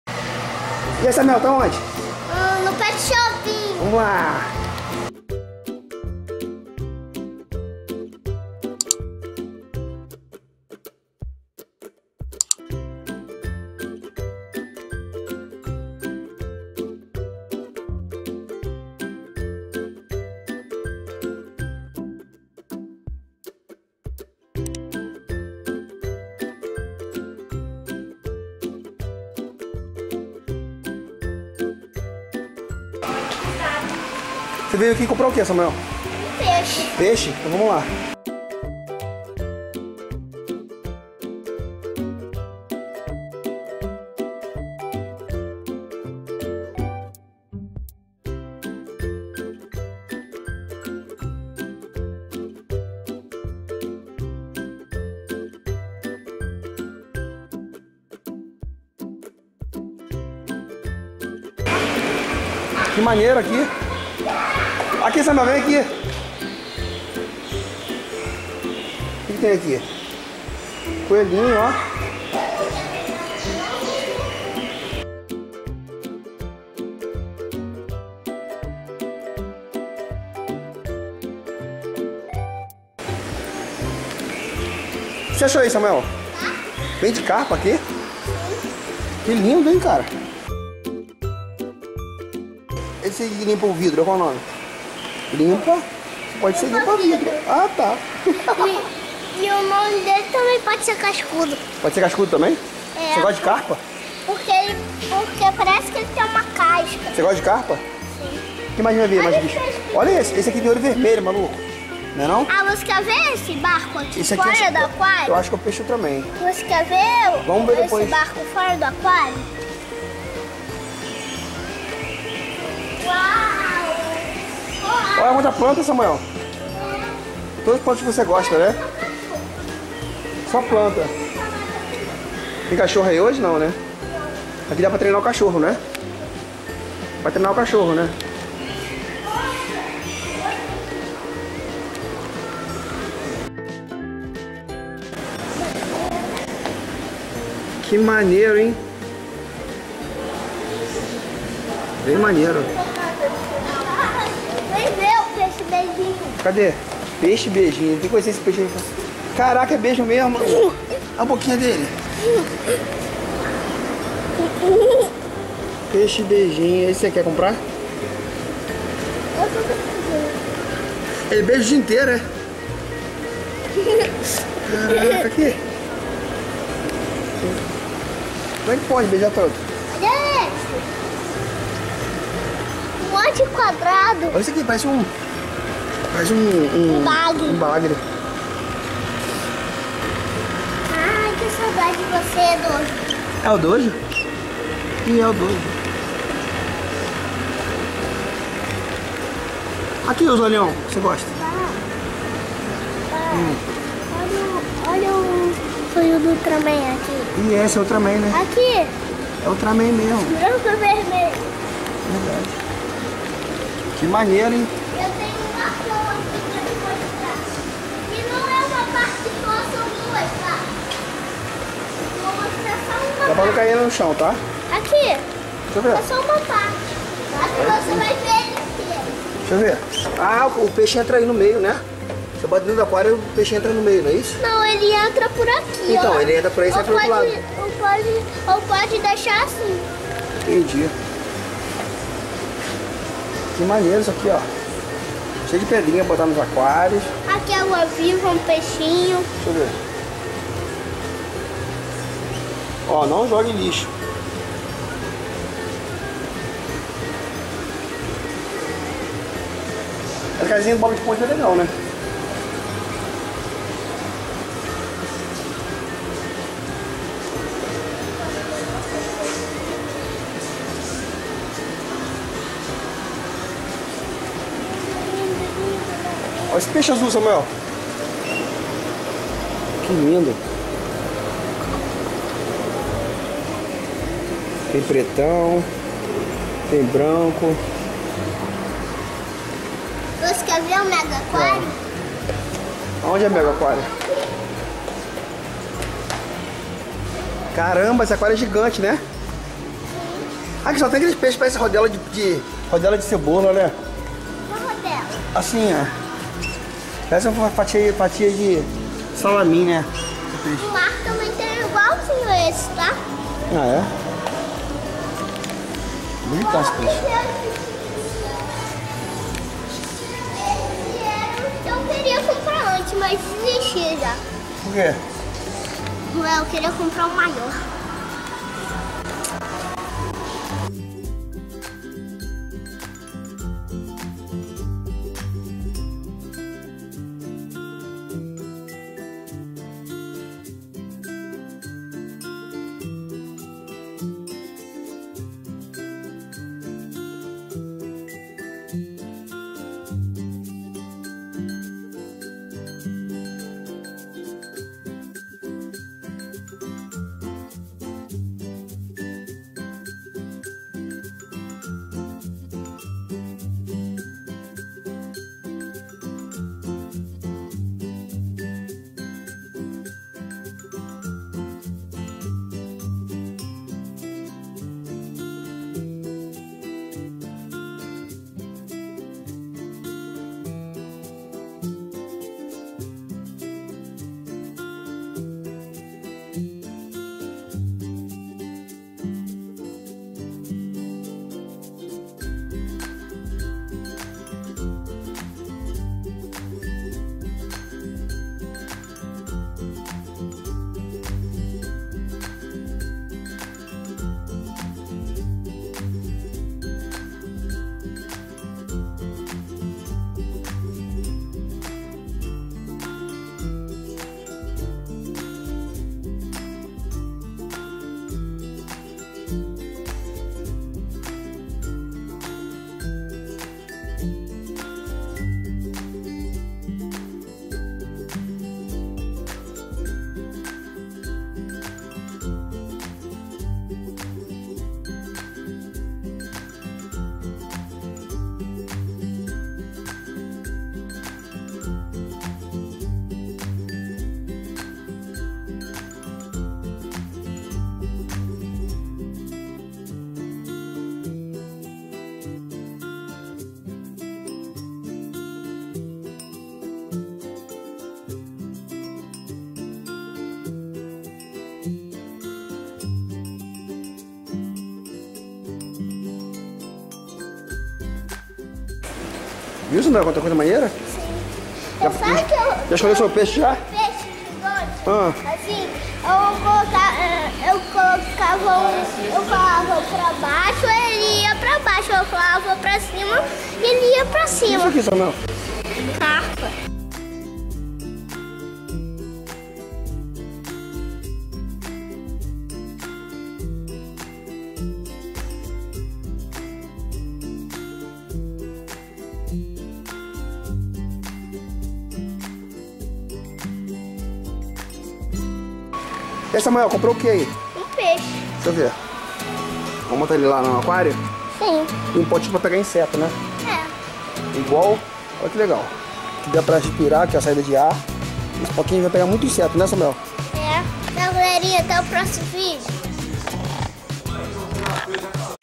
Comprei um Peixe Para o Meu Aquário
Visitei uma loja incrível de aquarismo e aproveitei e comprei um peixo!  Espero que goste do vídeo!
Obrigado!